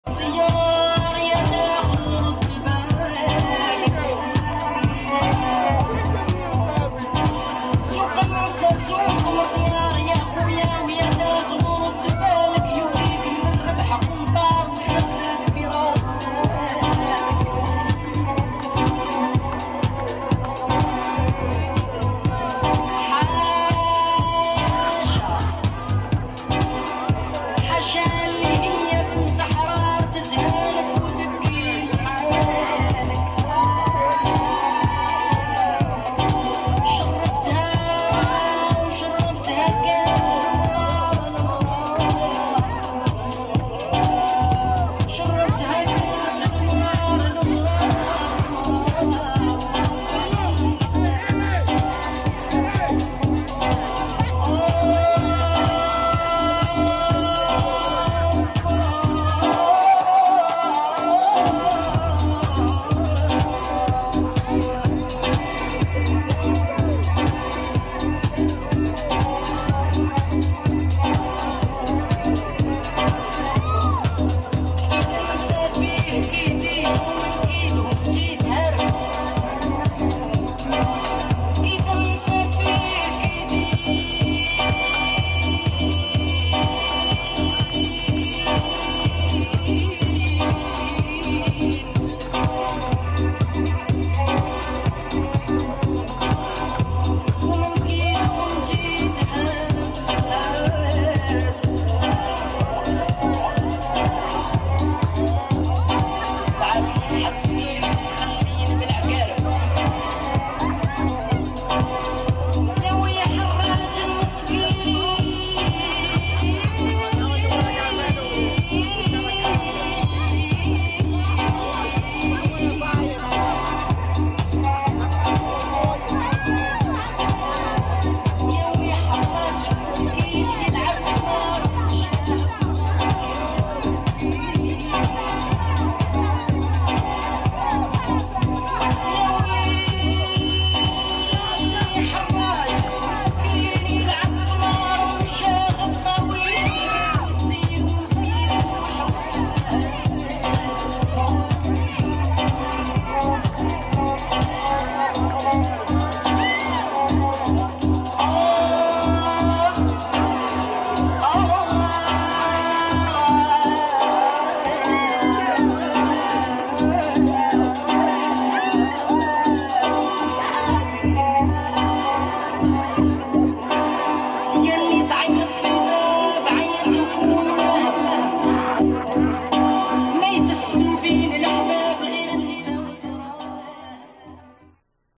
CONEY ISLAND BOARDWALK BEACH PARTIE 2 ( 8-20 - 11 )
WITH THE CONEY ISLAND DANCERS
ON THIS DAY KENNY BOBIEN SHOWED UP AND PUT ON A GOOD SHOW
 I THANK RICAN VARGAS FOR MAKEING  IT ALL HAPPIN THANKS